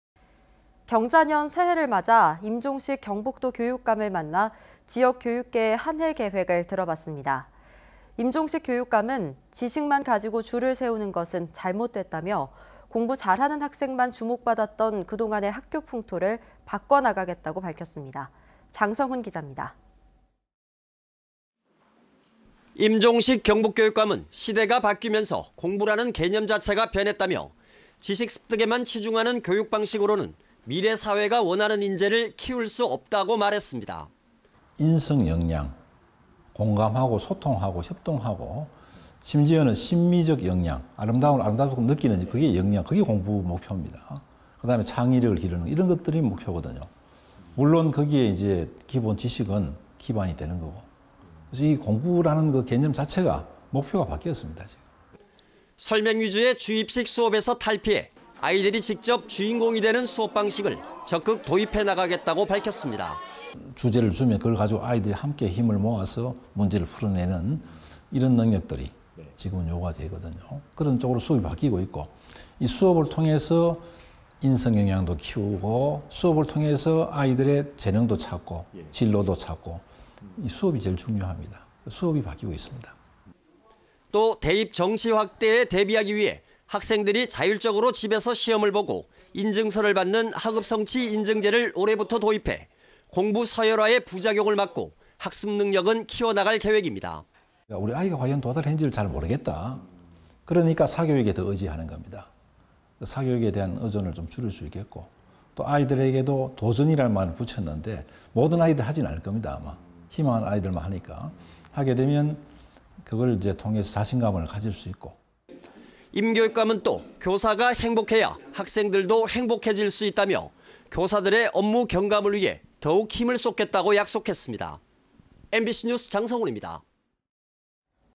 2020 임종식 교육감 신년대담-장성훈[포항MBC뉴스]
◀ＡＮＣ▶
경자년 새해를 맞아
임종식 경북도 교육감을 만나
지역 교육계의 한해 계획을 들어봤습니다.

임종식 교육감은
"지식만 가지고 줄을 세우는 것은 잘못됐다"며
공부 잘하는 학생만 주목 받았던
그동안의 학교 풍토를
바꿔 나가겠다고 밝혔습니다.

장성훈 기잡니다.
           ◀ＥＮＤ▶
           ◀ＶＣＲ▶

임종식 경북교육감은 시대가 바뀌면서
공부라는 개념 자체가 변했다며,

지식 습득에만 치중하는 교육 방식으론
미래 사회가 원하는 인재를 키울 수 없다고
말했습니다.

      ◀ＩＮＴ▶임종식/경북교육감
0725 "인성역량 공감하고 소통하고 협동하고 심지어는 심미적 역량, 아름다움을 느끼는 역량
그게 공부 목표입니다 그리고 창의력을 기르고 이런 것이 목표거든요 물론 거기에 기본 지식은 기반이 되는 거고..공부라는 개념 자체가 목표가 바뀌었습니다"

때문에 설명위주의 주입식 수업에서 탈피해
아이들이 직접 주인공이 되는 수업방식을
적극 도입해 나가겠다고 밝혔습니다.

 "주제를 주면 그걸 가지고 아이들이 함께 문제를 풀어내는 이런 능력들이 지금 요구가 되거든요  그런 쪽으로 수업이 바뀌고 있고 수업을 통해서 인성역량도 키우고 수업을 통해서 아이들의 재능도 찾고 진로도 찾고 수업이 제일 중요합니다 수업이 바뀌고 있습니다"

또 대입 정시 확대에 대비하기 위해
학생들이 자율적으로 집에서 시험을 보고
인증서를 받는 '학업성취인증제'를
올해부터 도입해,
공부 서열화의 부작용을 막고
학습 능력은 키워나갈 계획입니다.

1455 "우리아이가 과연 (학업성취 목표에)도달했는지 잘 모르겠다  그러니까 사교육에 더 의지하는 겁니다 사교육에 대한 의존을 좀 더 줄일 수 있겠고 아이들에게도 도전이란 말을 붙였는데 모든 아이들이 하지는 않을 겁니다 아마 희망하는 아이들만 하니까 하게 되면 그걸 통해서 자신감을 가질 수 있고.."

임 교육감은 또
"교사가 행복해야  학생들도 행복해 질 수 있다"며 교사들의 업무 경감을 위해
더욱 힘을 쏟겠다고 약속했습니다.

MBC뉴스 장성훈 입니다.